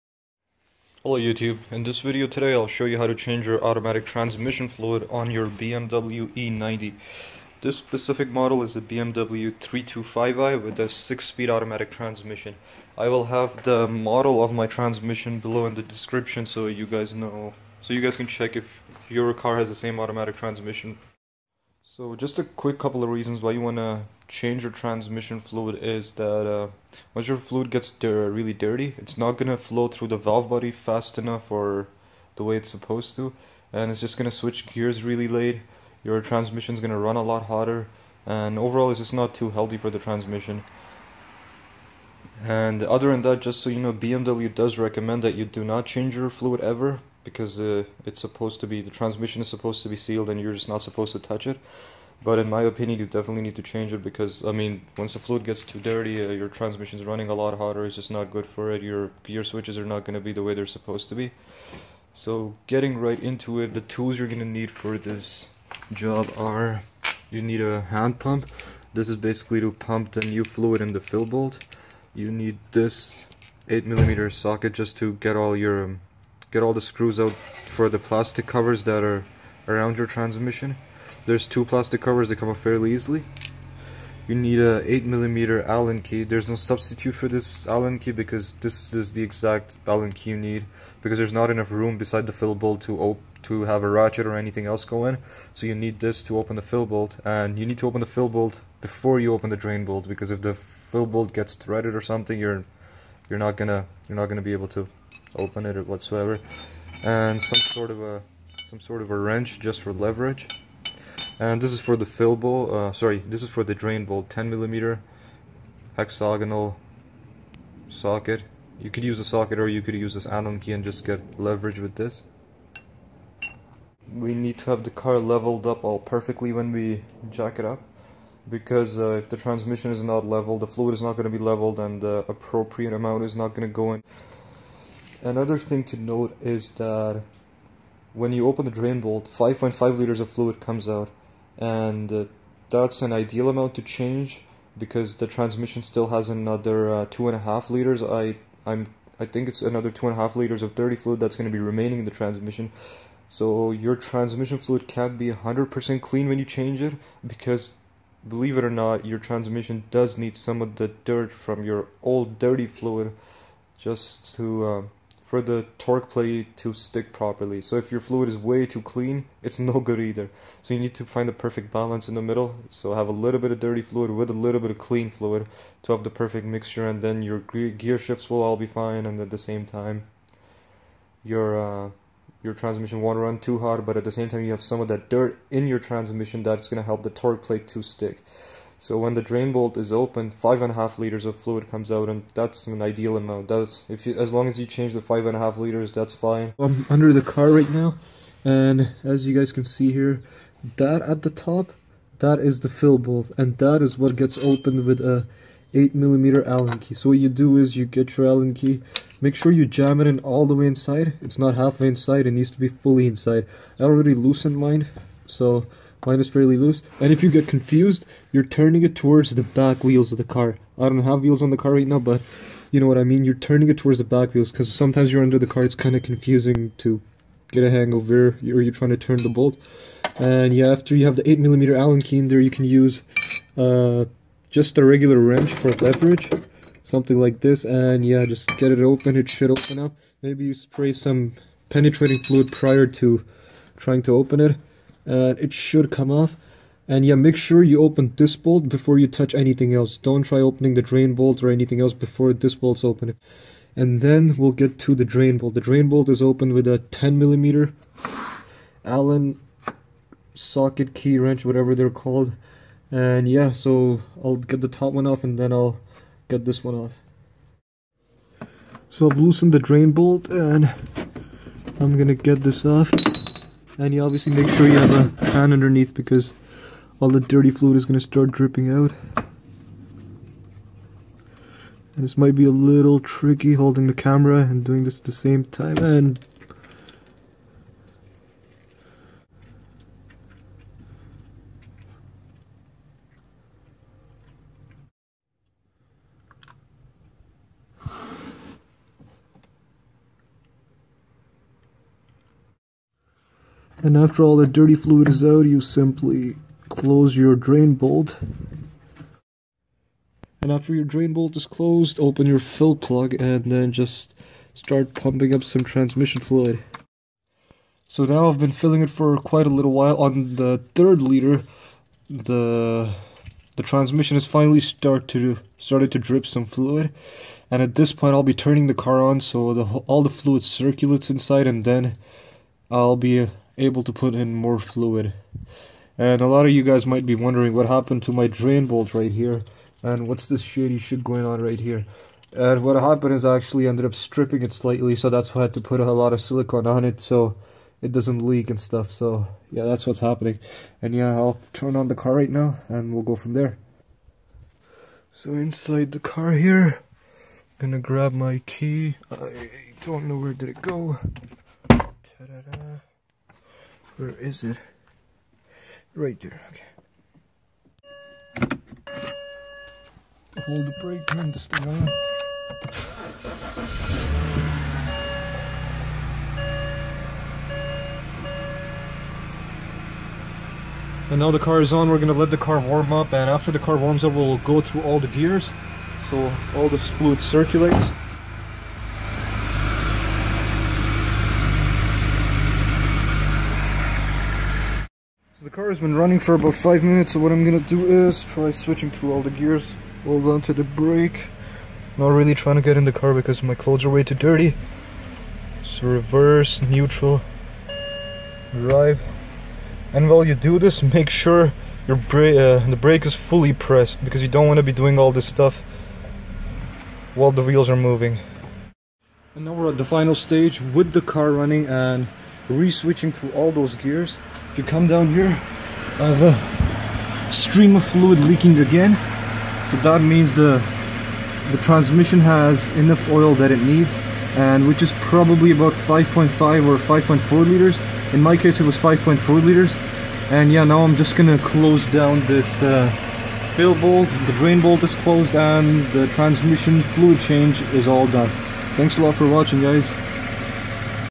2006+ e90/e92 BMW auto transmission fluid change
Tools needed 1:01
Fluid change begins at 3:15

This video gives a quick demonstration on how to change your automatic transmission fluid (not filter) on a 2006 bmw e90 325i. The same procedure works on a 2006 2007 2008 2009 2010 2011 BMW e92/e90 3 series 328i 330i and 335i.

My video only shows the fluid change, not the filter and transmission gasket change

Transmission model: ZF GA6HP19Z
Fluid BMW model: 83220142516